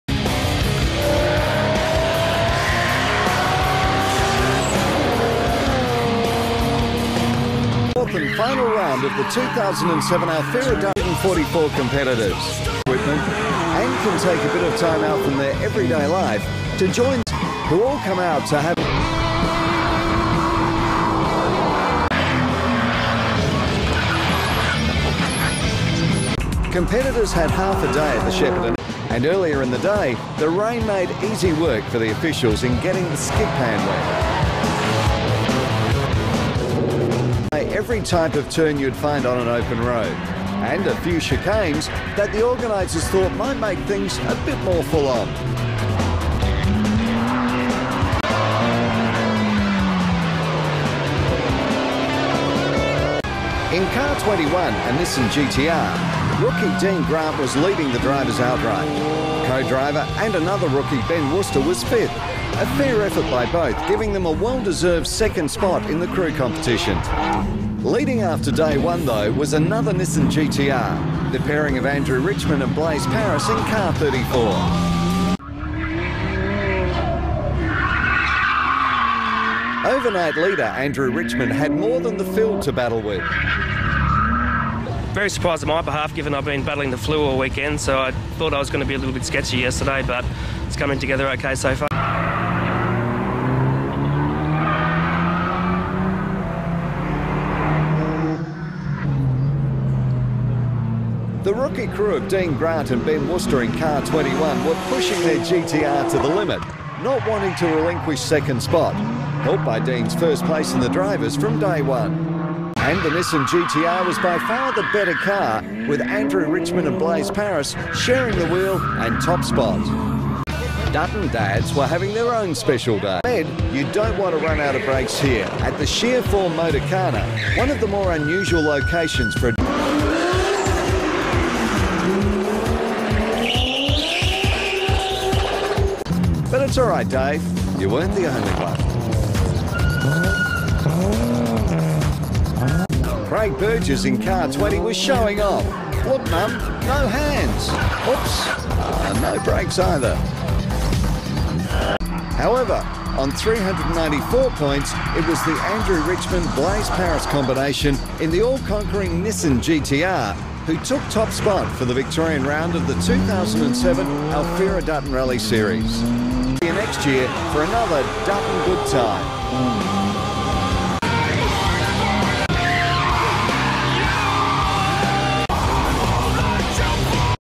Dutton Rally Vic - Highlights
Highlights of the TV coverage of the 2007 Dutton Rally held in Victoria.